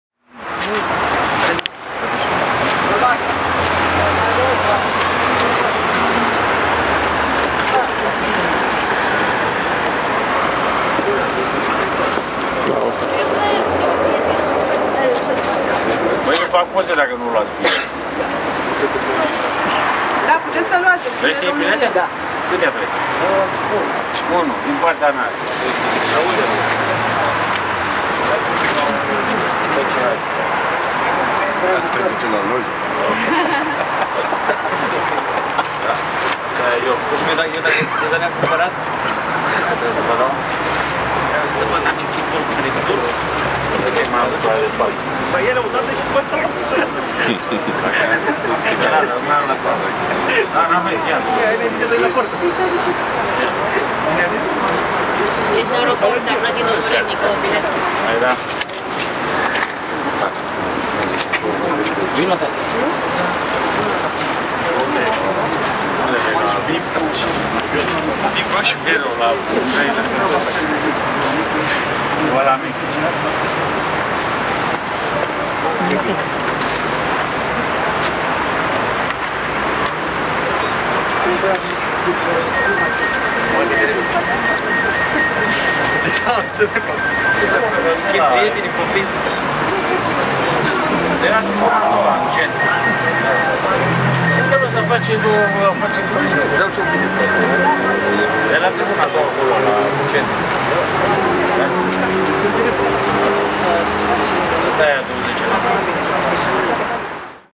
Mircea Rednic a vandut bilete suporterilor
Mircea Rednic a fost prezent la casele de bilete de la stadionul Dinamo, pentru a vinde tichete fanilor dinamovisti